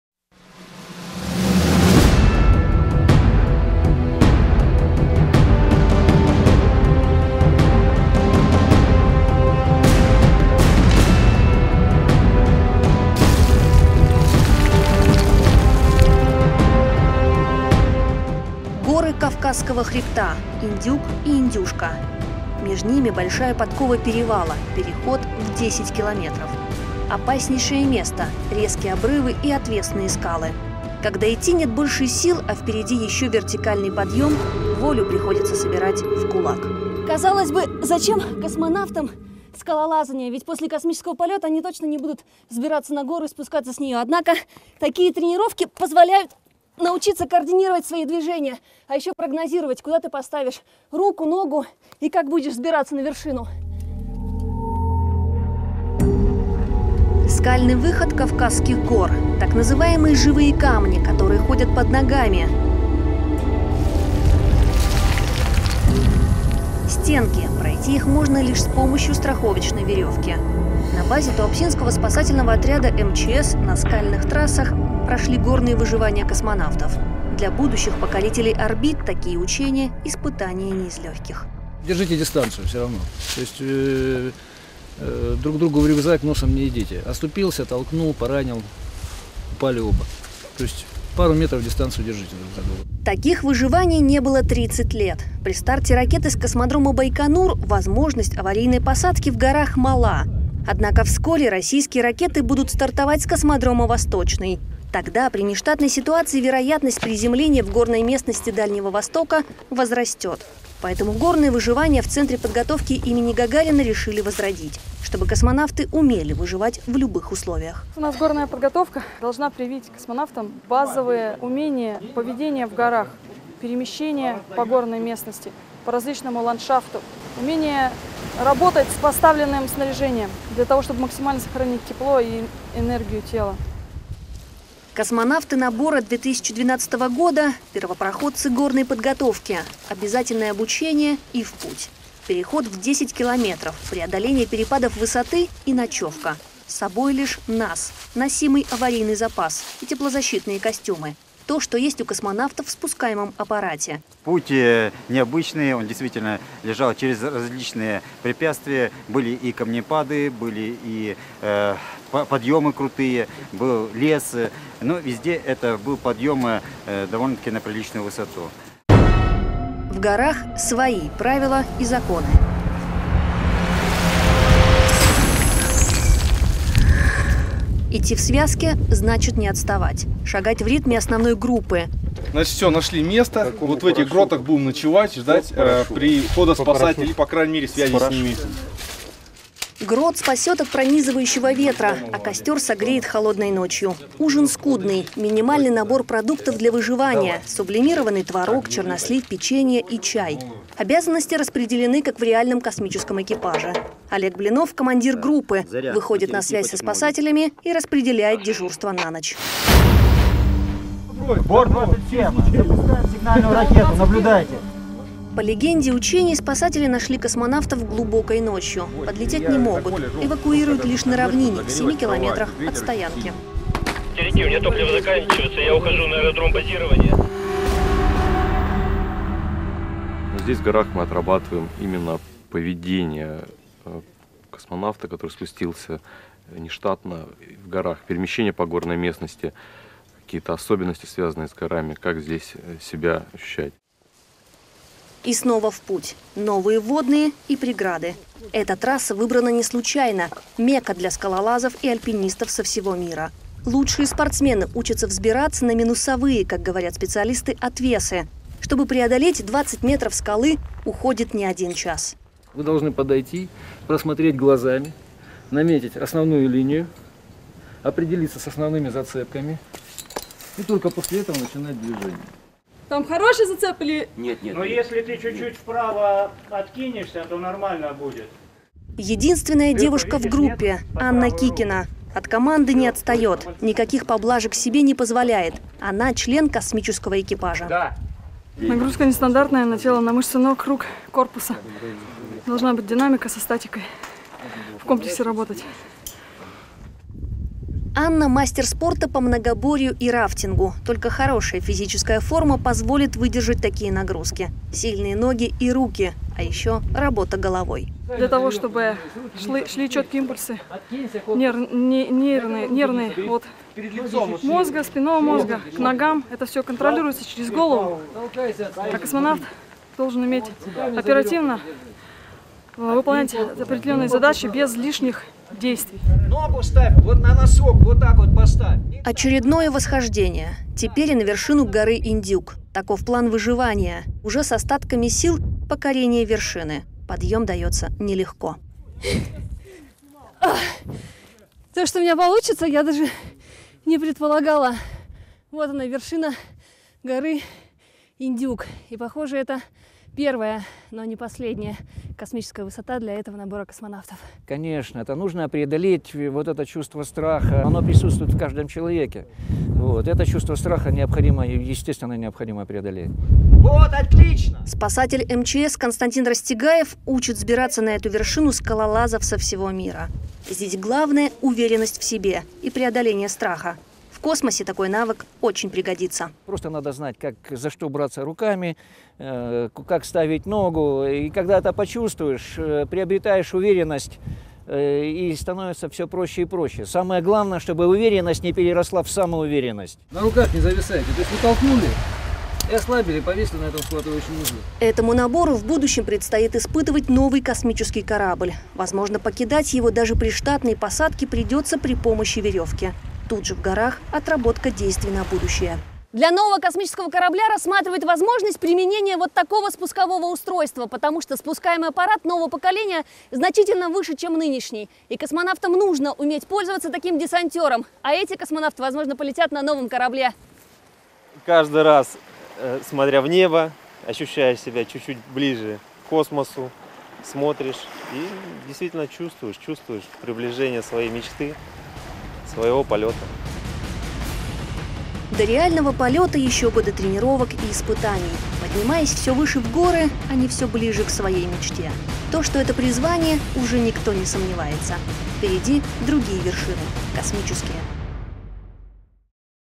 Космонавты - скалолазы
К выживанию в пустыне, в зимнем лесу и на воде добавился еще один вид испытаний для космонавтов. Теперь будущие покорители орбит отрабатывают действия при нештатном приземлении в гористой местности. Это испытание не просто проверка на выносливость. Траектория будущих пилотируемых пусков с космодрома «Восточный» будет частично проходить над горами.